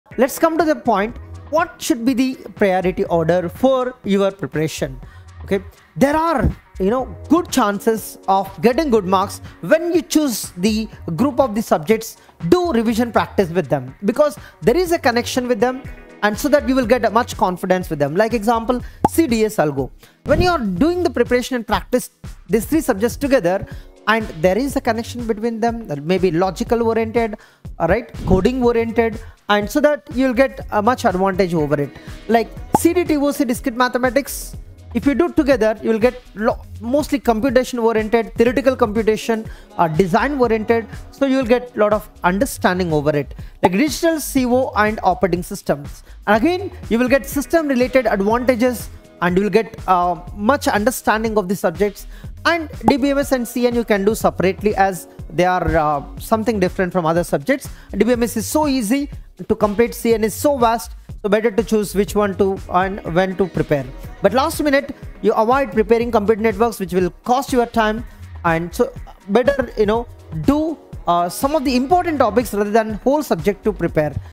What Should be your PRIORITY list to complete syllabus for GATE 2026
Confused about what to study first and what to skip while preparing for GATE 2026? In this video, we discuss the ideal PRIORITY list to complete the GATE 2026 syllabus, focusing on high-weightage subjects, scoring topics, and dependency-based order. You’ll learn how toppers decide subject priority, how to balance core subjects, mathematics, and aptitude, and how to plan syllabus completion smartly without burnout. This video is a must-watch for GATE 2026 aspirants who want maximum output with limited time.

📕 𝐁𝐚𝐭𝐜𝐡/𝐂𝐨𝐮𝐫𝐬𝐞 𝐋𝐢𝐧𝐤𝐬:-

 GATE DA Weekday / Weekend

 GATE SPL. Combo Course - CSIT + DA

👍 Like | 🔄 Share | 📢 Spread the word